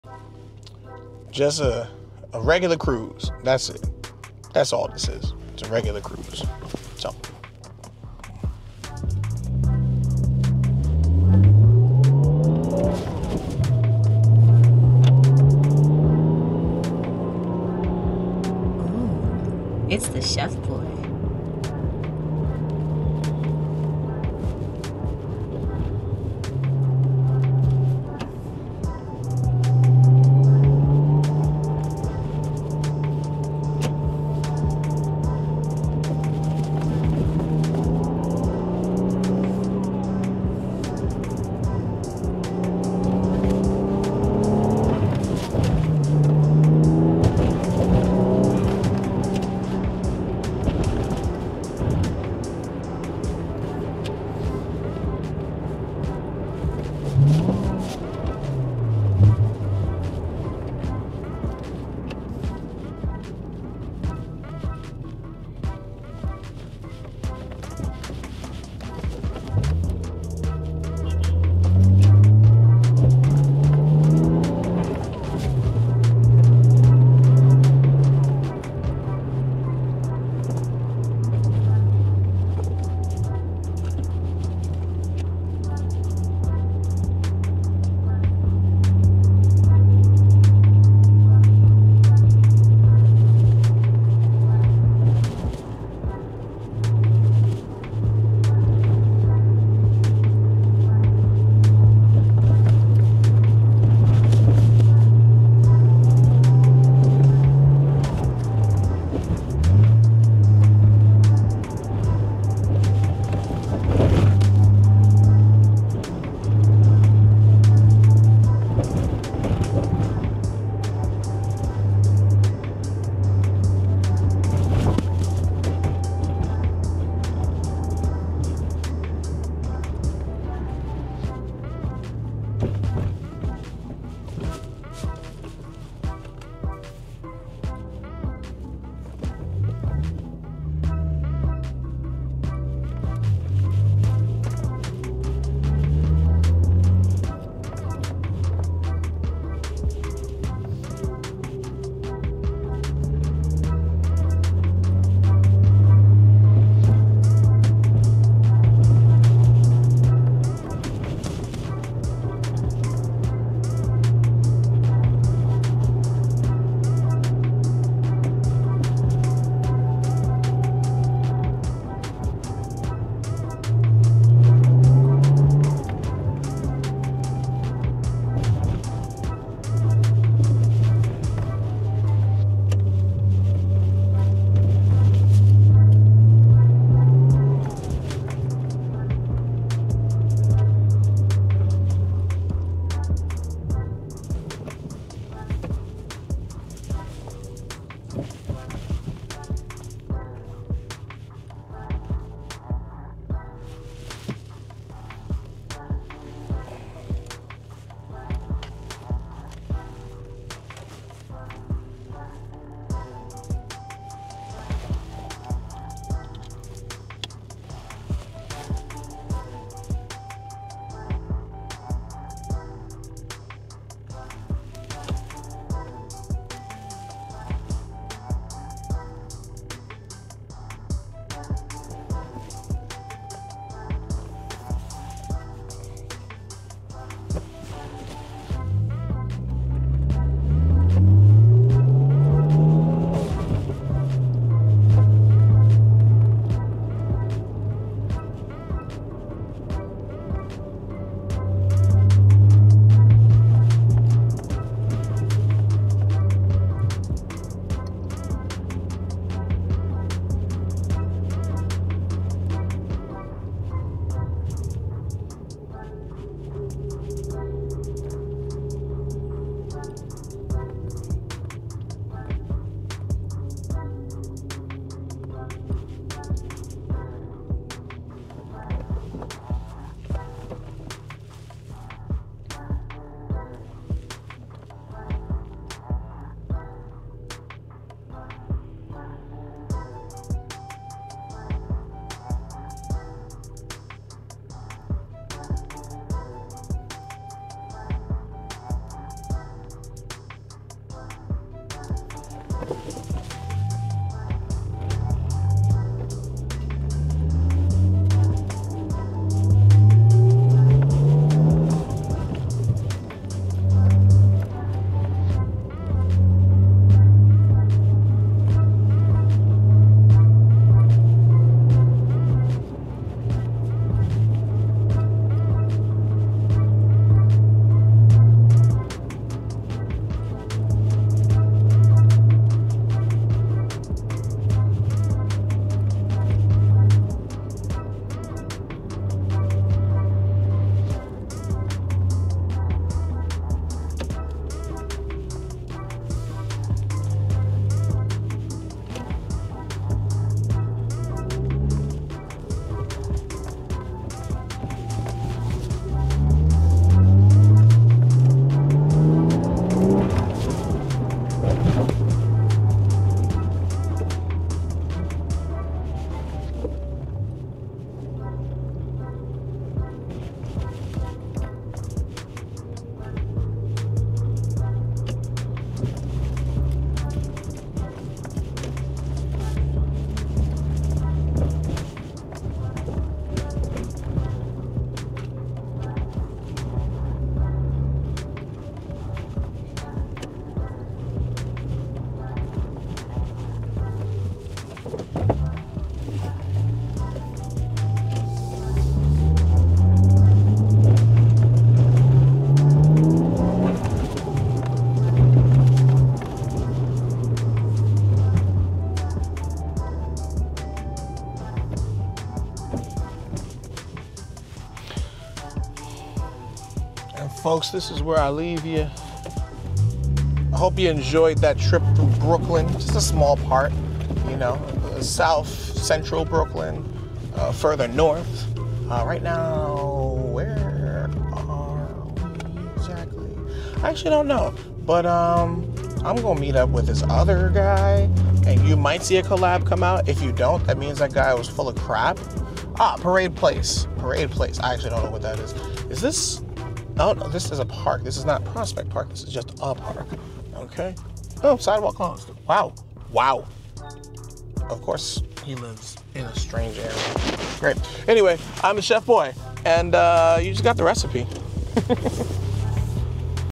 JDM POV in BK NY - 93 Toyota Chaser
Join me for an exhilarating POV drive through the vibrant streets of Brooklyn, NY, in a classic 1993 Toyota Chaser Tourer V! Experience the unique thrill of this right-hand drive, manual 5-speed beauty equipped with a powerful 1JZ engine.

In this video, you’ll enjoy breathtaking views of Brooklyn’s iconic neighborhoods, bustling streets, and hidden gems—all from the driver’s seat of this legendary JDM sports sedan. Whether you're a car enthusiast, a fan of classic vehicles, or simply curious about life on the road in New York City, this video is a must-watch!

What You’ll See:

Stunning Brooklyn landscapes and street scenes
The smooth performance of the 1JZ engine
Insights into driving a right-hand drive vehicle in the U.S.
A nostalgic trip back to the ’90s with this iconic Toyota

Gear used to create this greatness:
DJI Pocket 3
DJI Mic 2